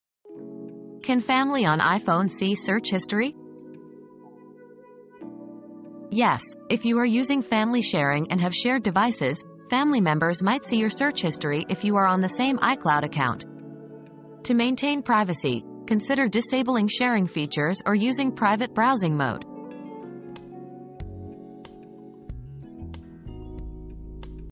Can family on iPhone see search history?
Protect Your Privacy: Prevent Family from Viewing Search History on iPhone • Privacy Protection Tips • Learn how to keep your search history private on shared devices. Disable sharing features or use private browsing mode to prevent family members from seeing your online activity.